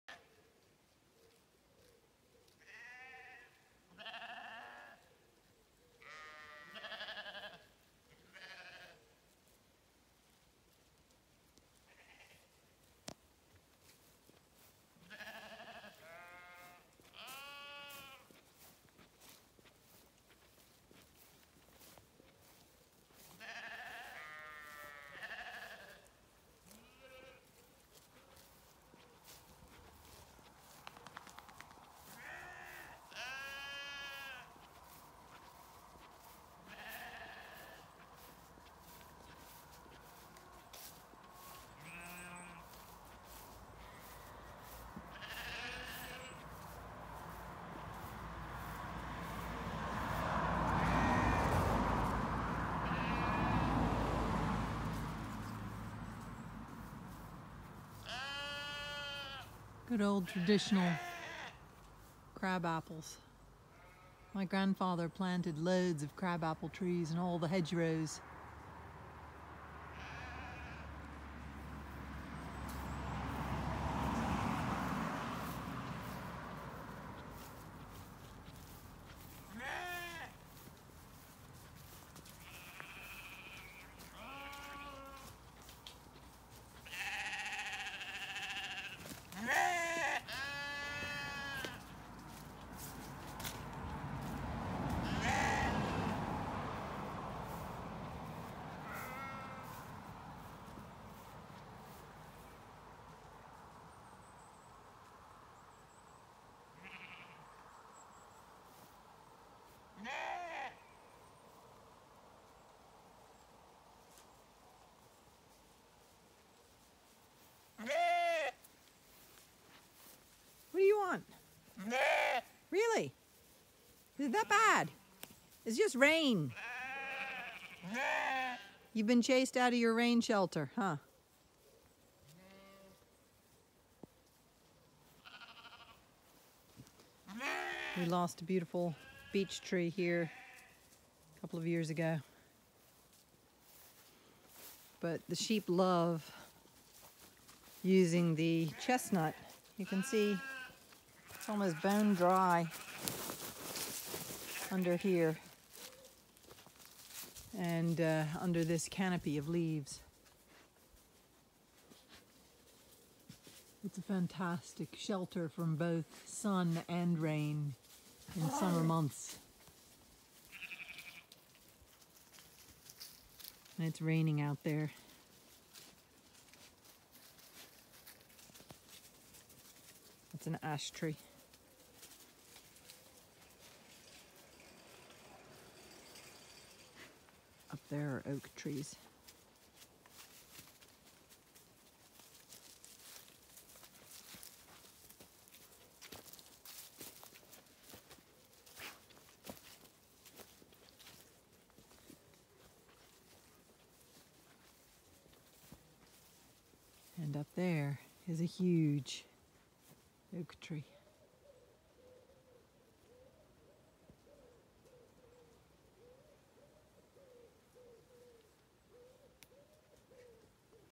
Walk under rain sheltering trees to look at sheep
Crap apples are fattening up. Horse chestnut trees I find are like umbrellas. They shelter me from the worst of this evening’s rain as I view the sheep up the hill.